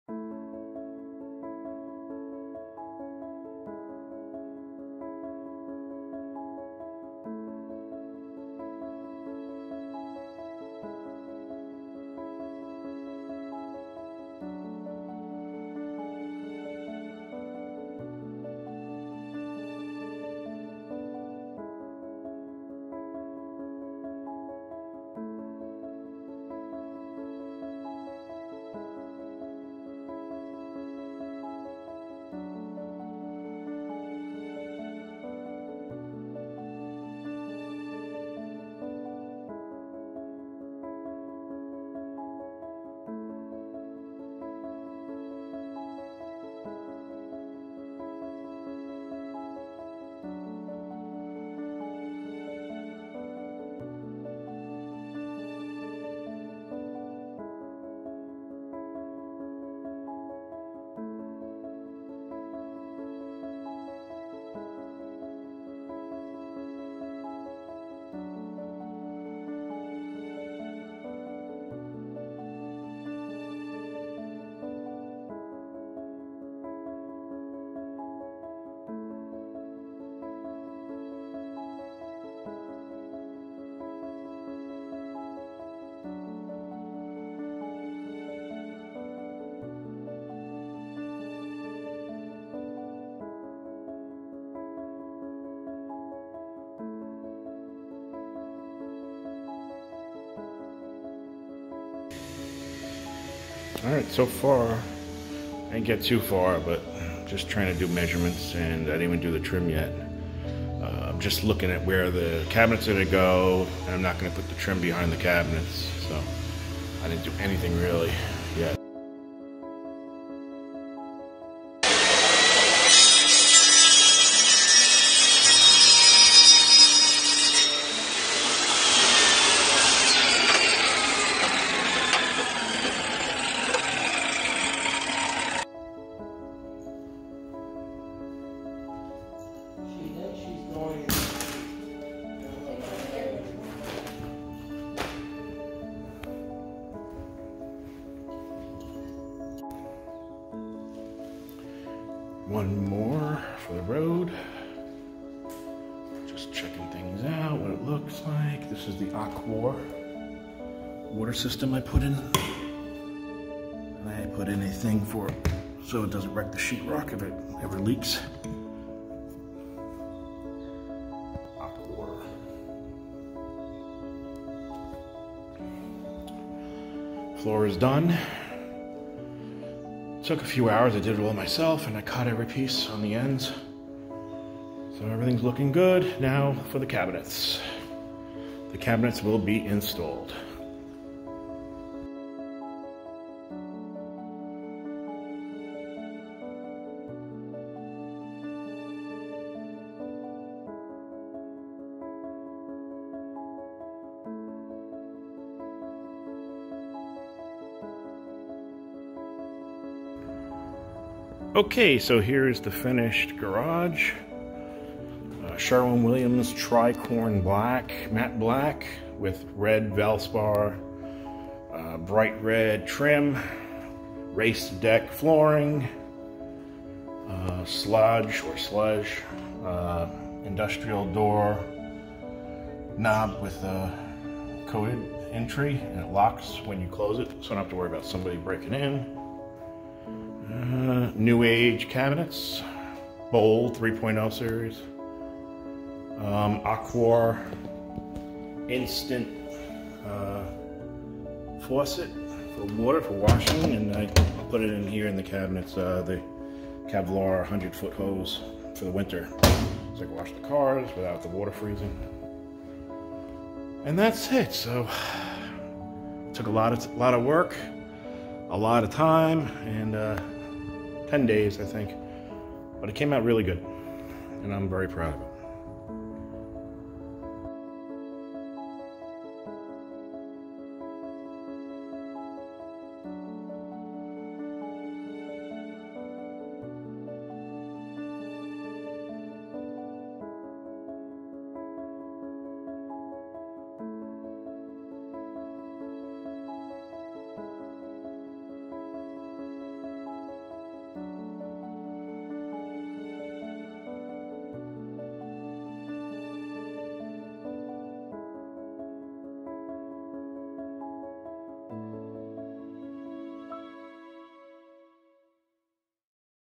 Racedeck flooring and New Age cabinets 3.0 garage upgrade
I used a graco 360 dual speed airless paint sprayer. aquor instant water faucet. new age bold 3.0 cabinets with stainless steel. schlage flex lock door handles. Special thanks to racedeck and new age for the great discounts after I kept bothering their salesmen LOL